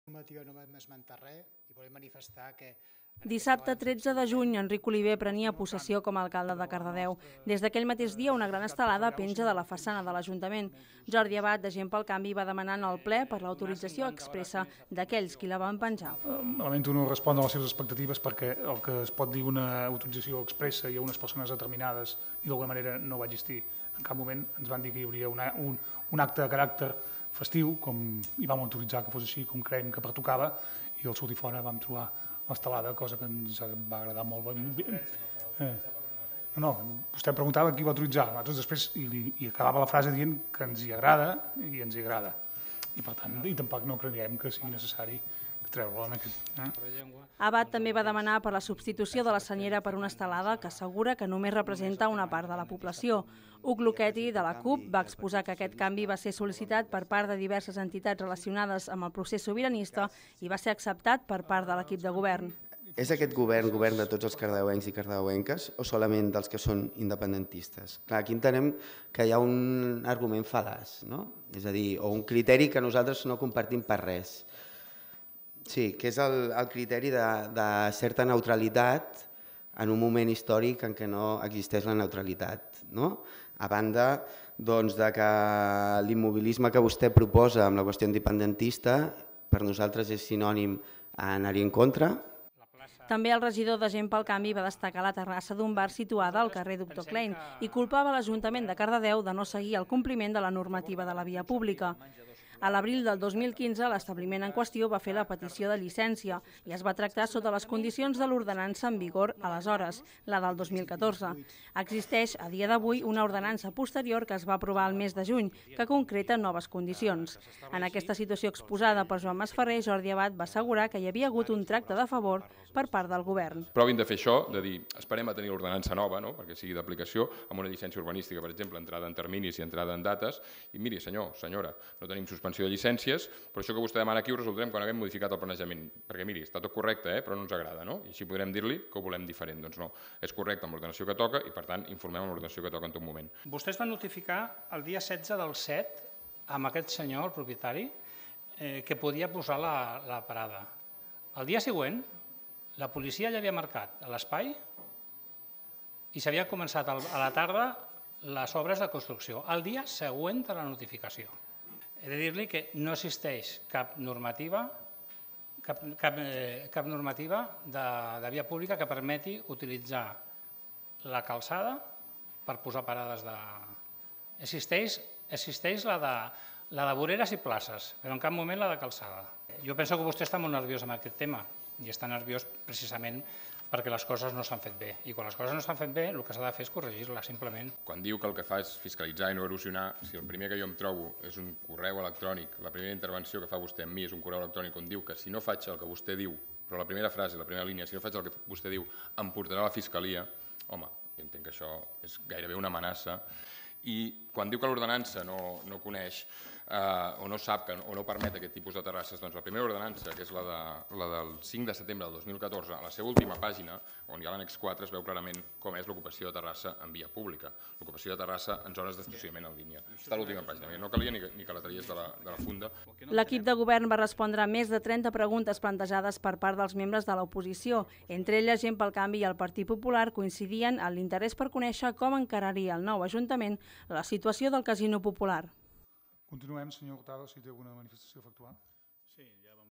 L'equip de govern respon més de 30 preguntes de l'oposició
L’equip de govern va haver de respondre a més de 30 preguntes de la oposició en el ple municipal del mes de juliol. Entre elles, van sortir temes com la Festa Major o la gestió del Casino Popular. A continuació comentem dues temàtiques més: les banderes de l’Ajuntament de Cardedeu i la normativa reguladora de l’espai públic.